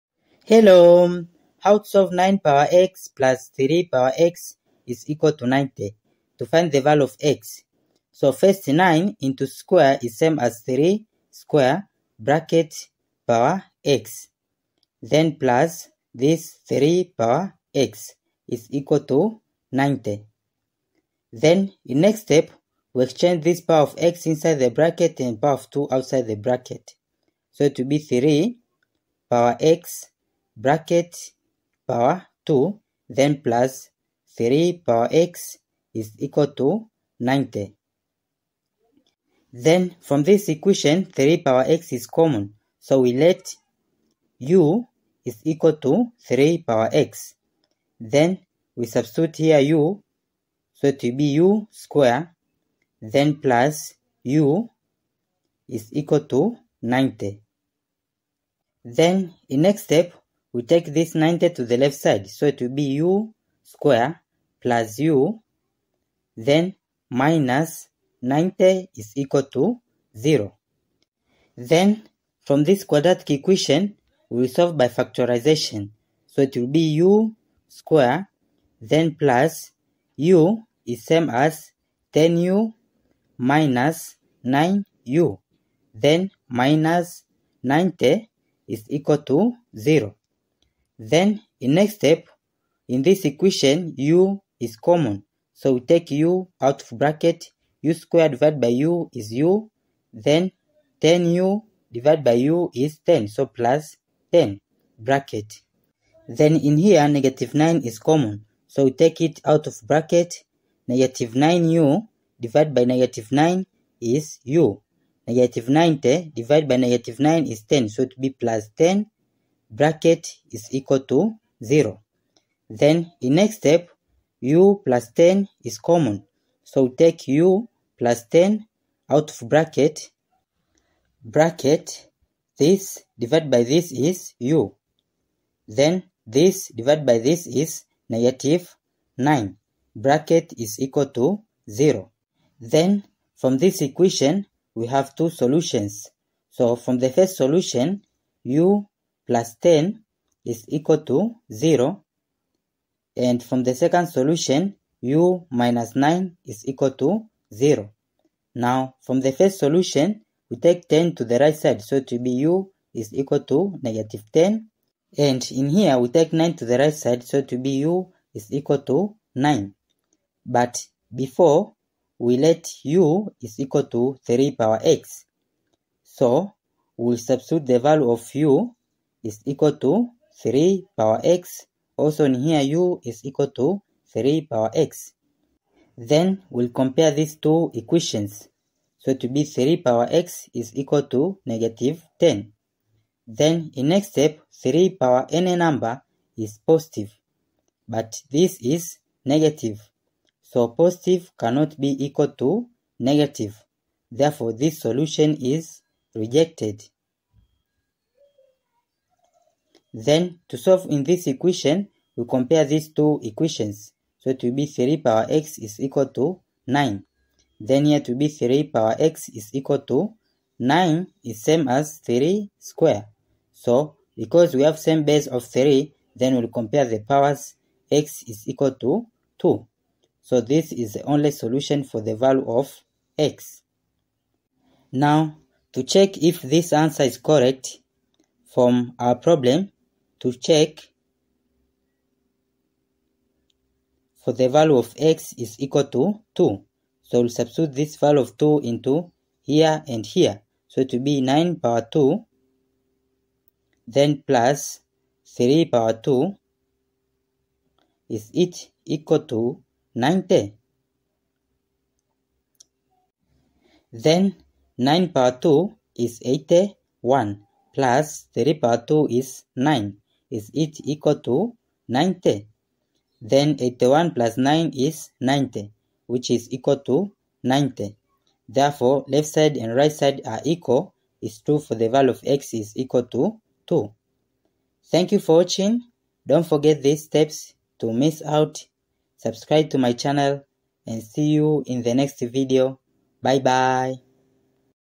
A Nice Olympiad Algebra Equation
How to solve for X in this maths Olympiad Algebra problem